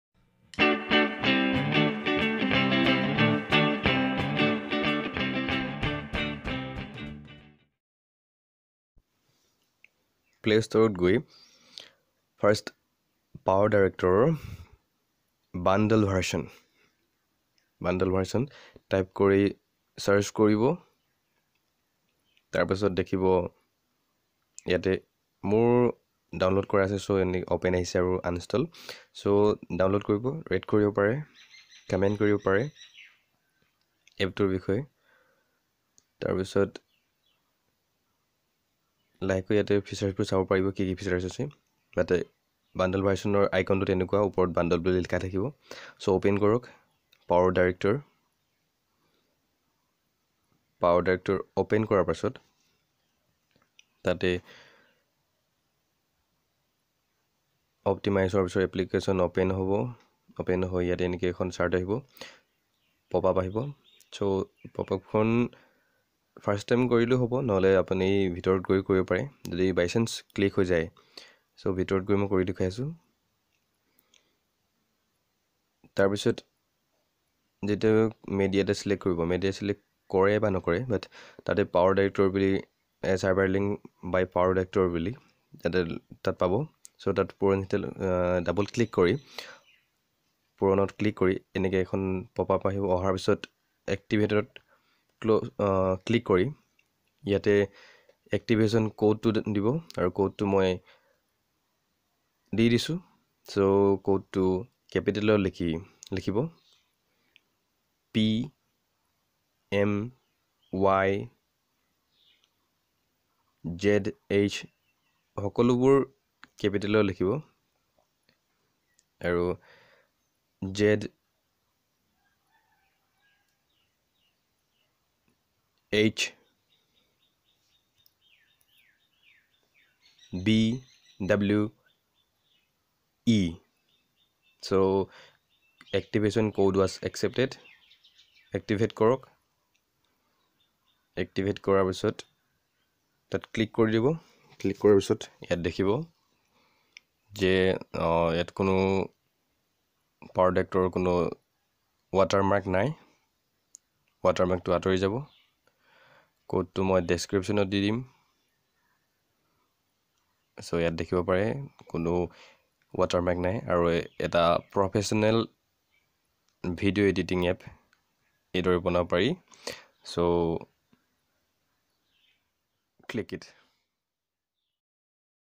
Watermark নাইকিয়া কৰক(Assamese)||Powerdirector||No root||RNC
In the video,I have explained how to delet the watermark of Poerdirector (BundleVersion) by using a activation code which 100% works.it helps you to create awesome videos like professional video editing app!
Activation Code : PMYZHBWE
Just try it and be a editor. !!